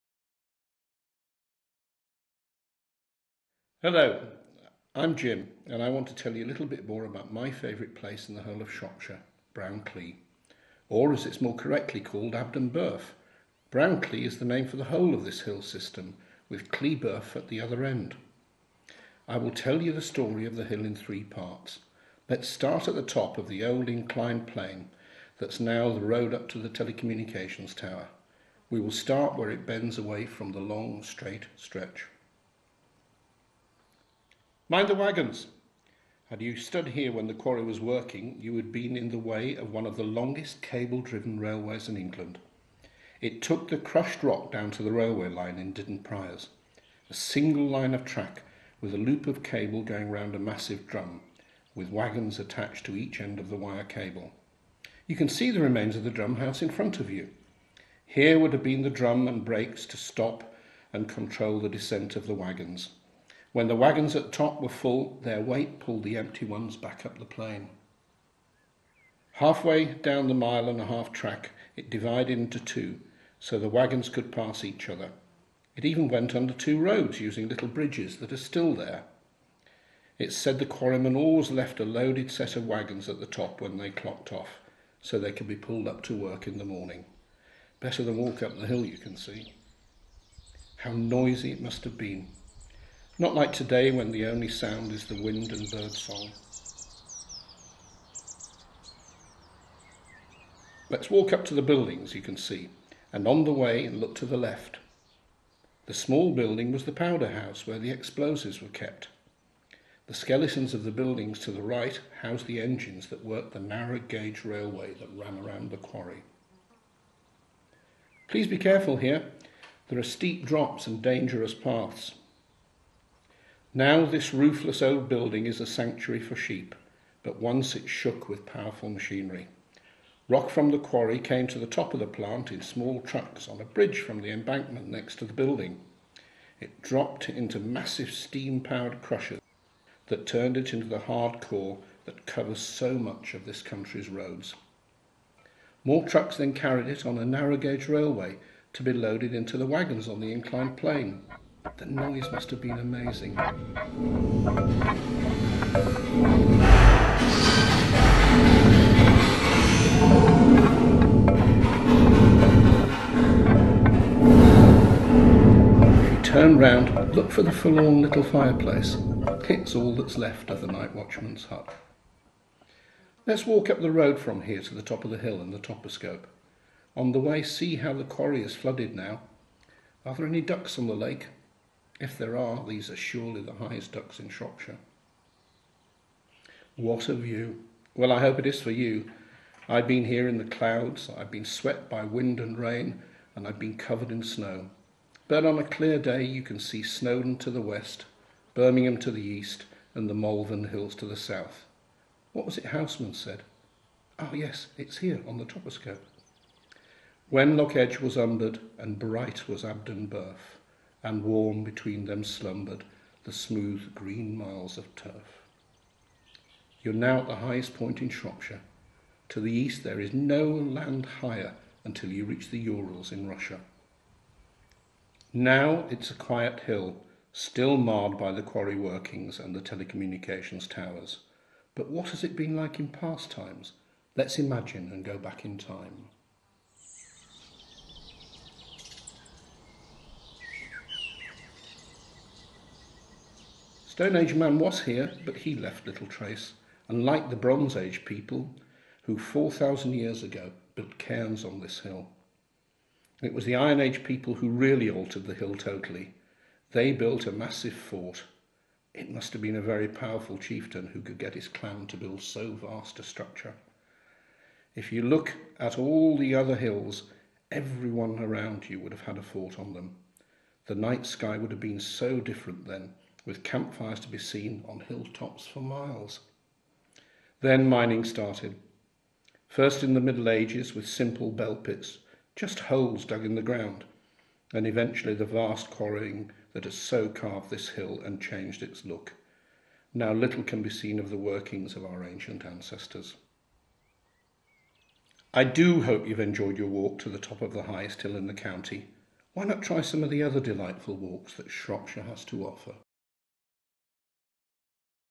Brown Clee with Jim Shropshire Way
Brown Clee with Jim Shropshire Way podcast  (  Audio only  )

found this download on my computer from the website above. thought i would share and promote the website above for anyone interested in more information about walking in Shropshire. PDF Audio guides and more are available.